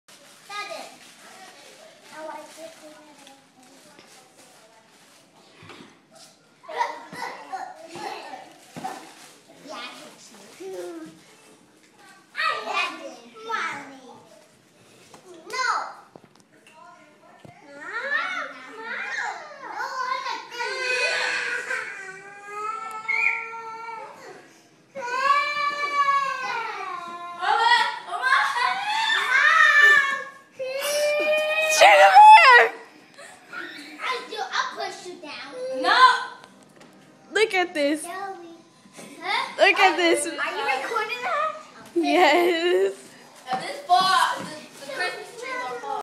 Baby's fight girl stuck on stair case
Baby's fight about nothing girls stuck on stair case for a whileee